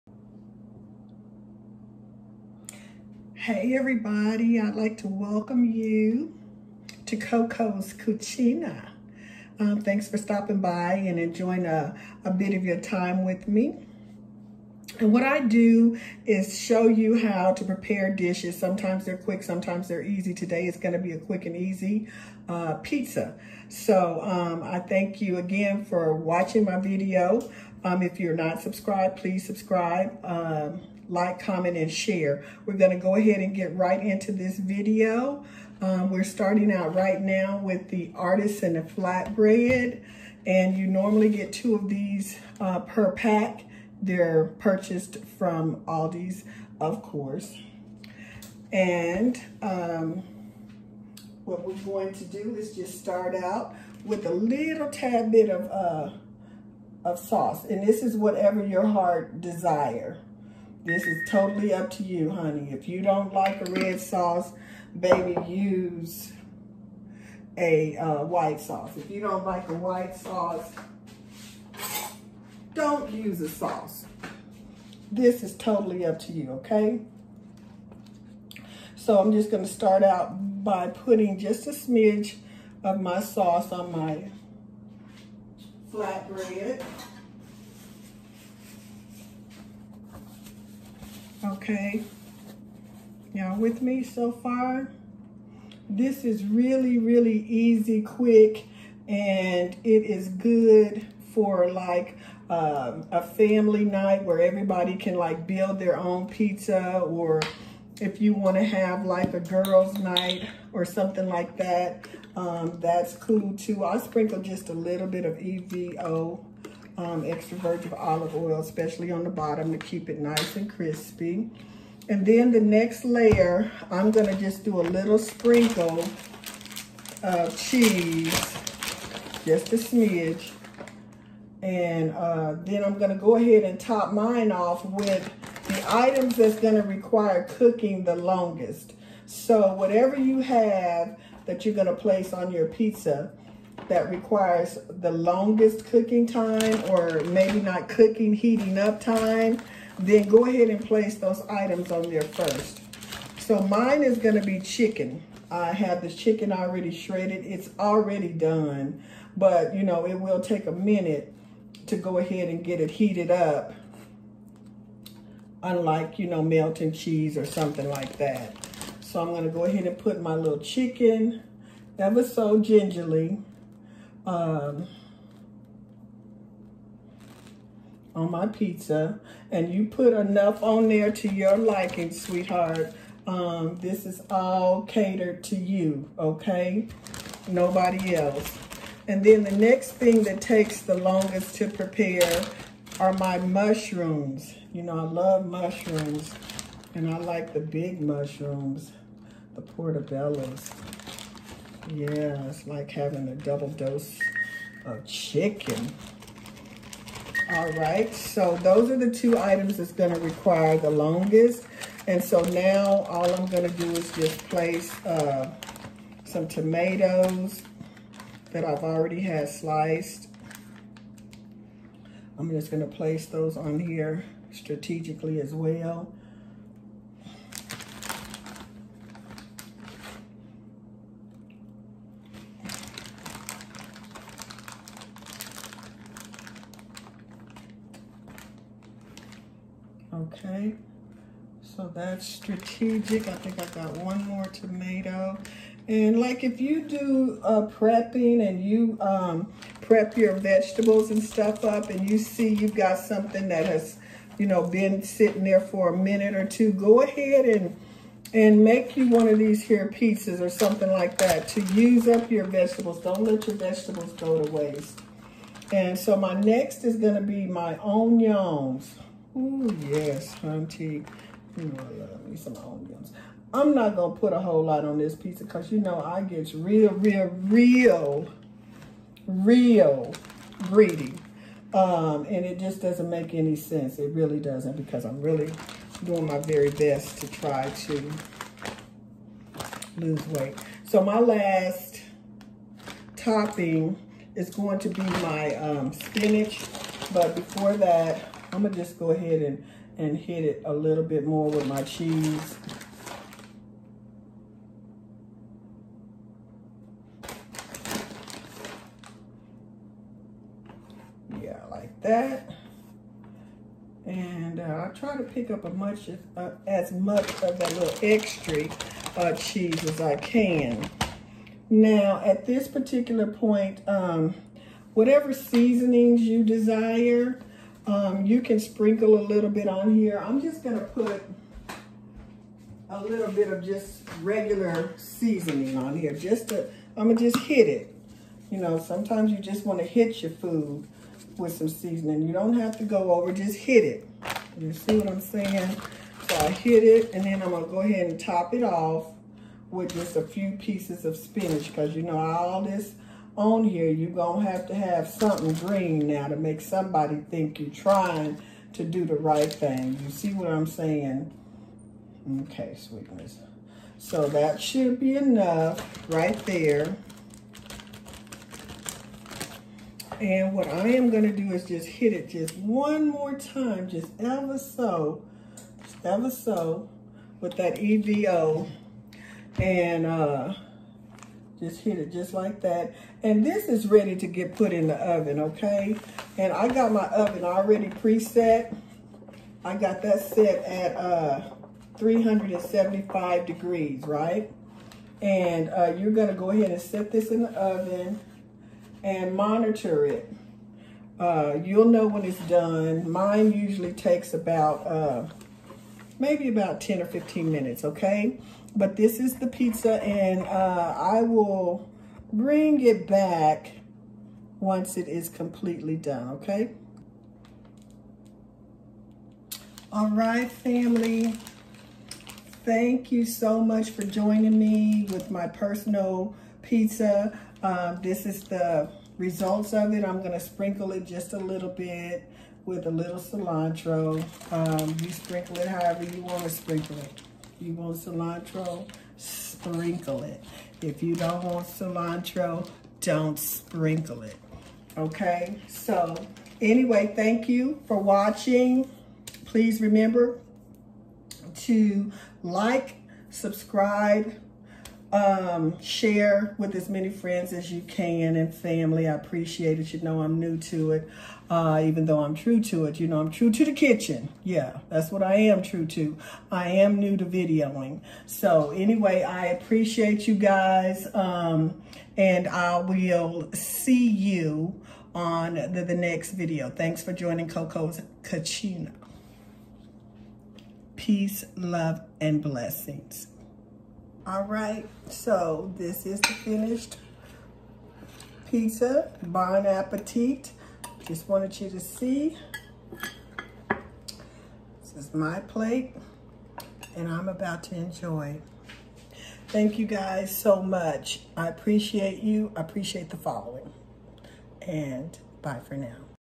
How to prepare Pizza the quick easy way.
Personal Pizza good for family /girls night build your own.
Peace, Love and Blessings
Ms Coco Unique Mitchell💋